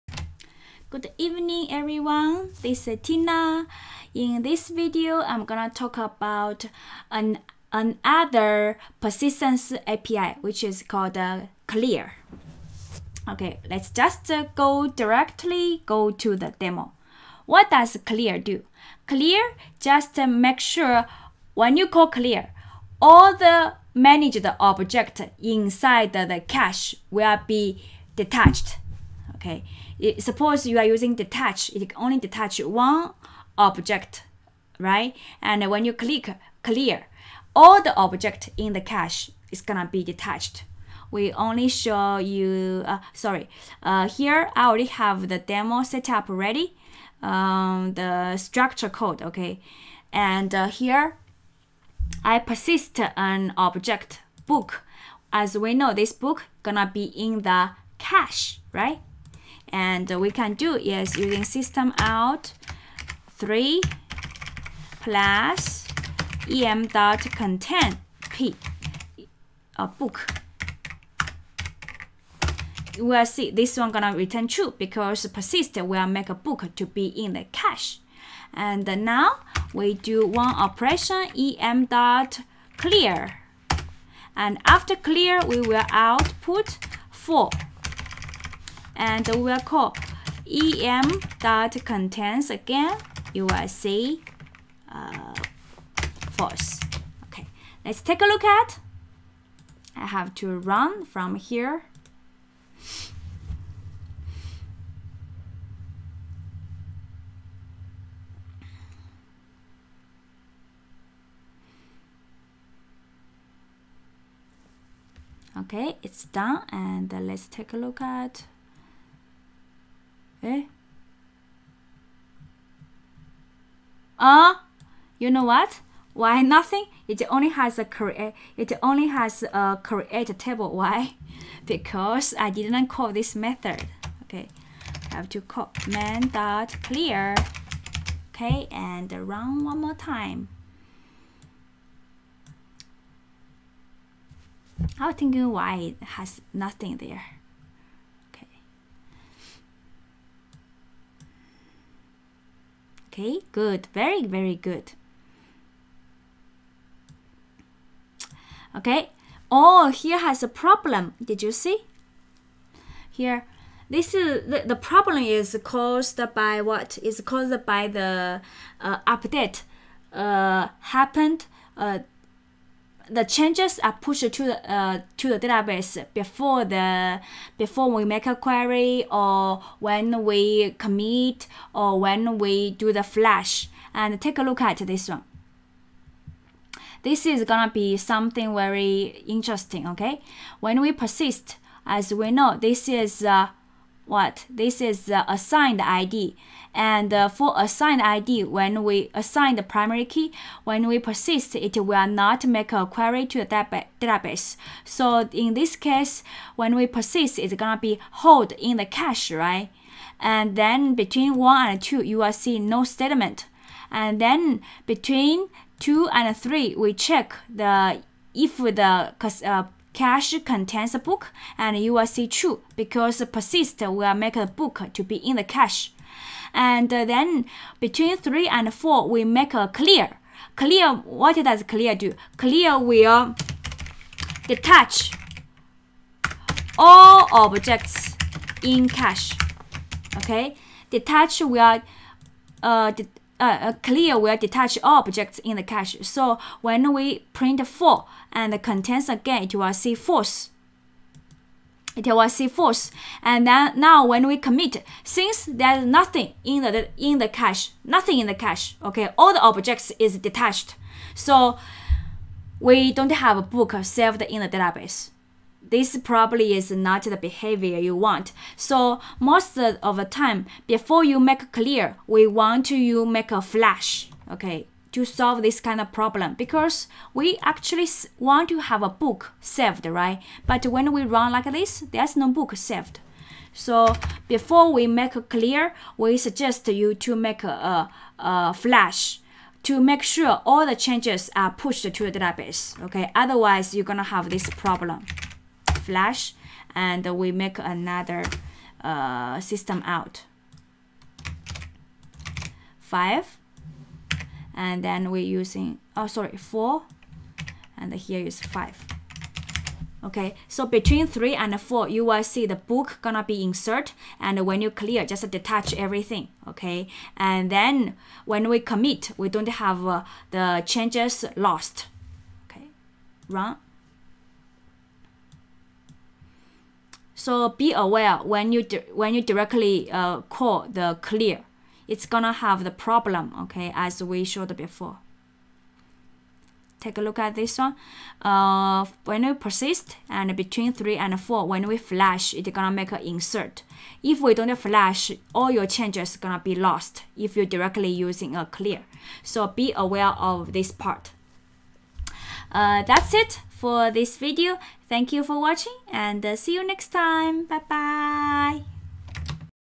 Persistence APIs - .clear()
This video discuss the usage of .clear() method which removes all entities from the cache. But make sure you do .flush() before call .clear(), otherwise, all your changes in cache will be lost.